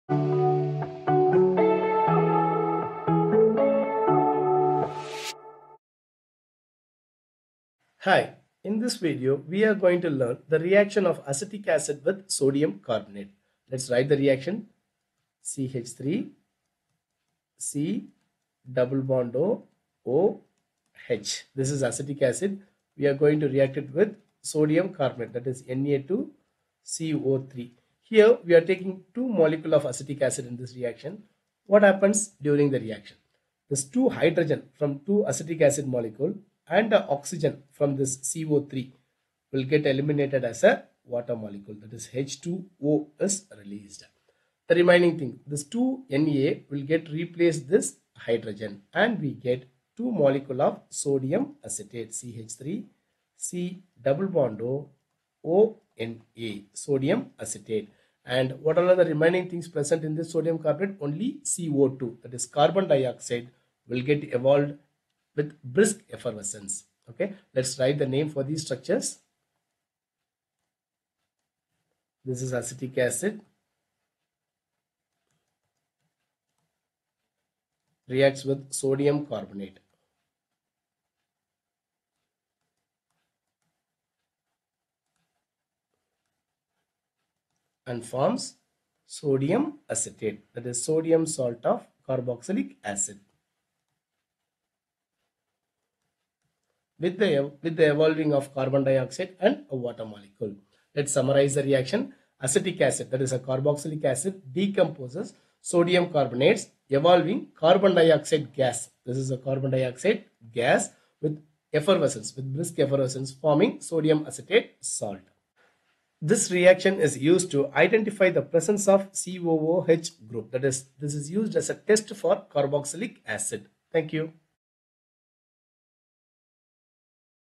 Reaction of Carboxylic Acid with Sodium Carbonate| Acetic Acid| Sodium Acetate| Organic Chemistry
The Carboxylic acids decompose carbonates and bicarbonates evolving carbon-dioxide gas with effervescence. Here Acetic acid reacts with sodium carbonate and forms sodium acetate with brisk effervescence of carbon-dioxide.